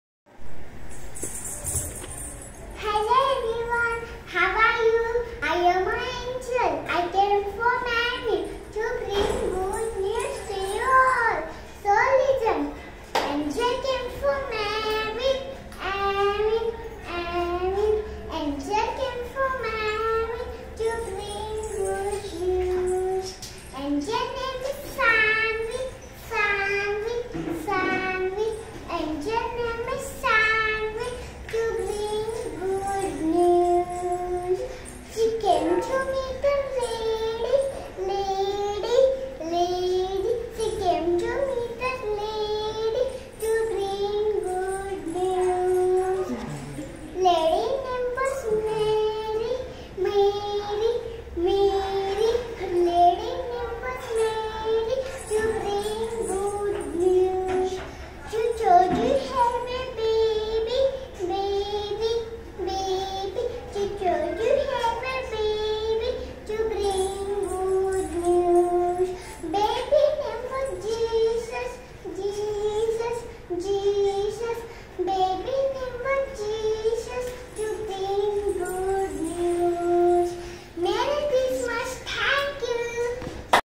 Fancy Dress competition on Christmas - Sanvi | kids | angel
Hello everyone 😊

I dressed up as angel in school competition on Christmas day and got 1st prize 🏆